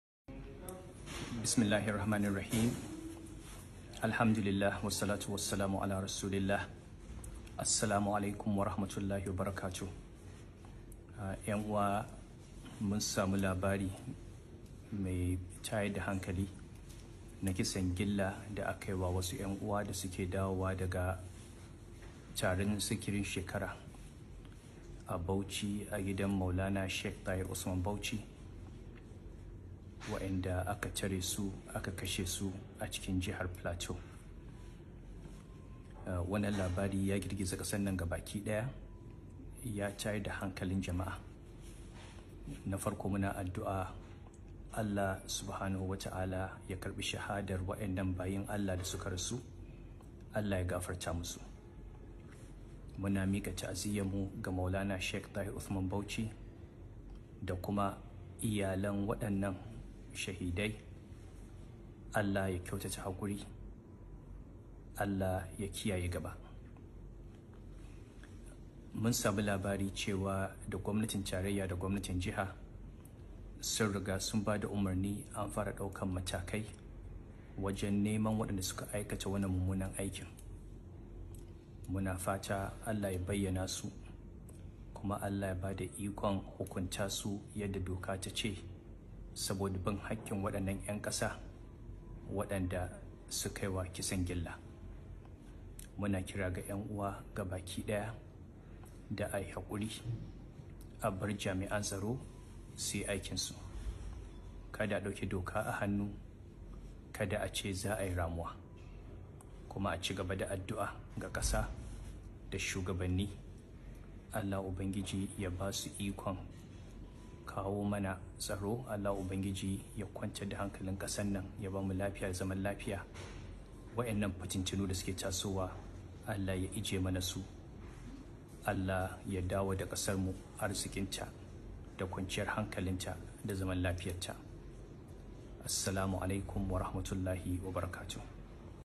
Ta'aziyyar Khalifa Sunusi Lamido Sanusi Kenan Ga Sheikh Dahiru Usman Bauchi RTA Tareda Jan Kunne
Ta'aziyyar Khalifa Sunusi Lamido Sanusi Kenan Ga Sheikh Dahiru Usman Bauchi RTA Tareda Jan Kunne Ga Yan uwa tijjanawa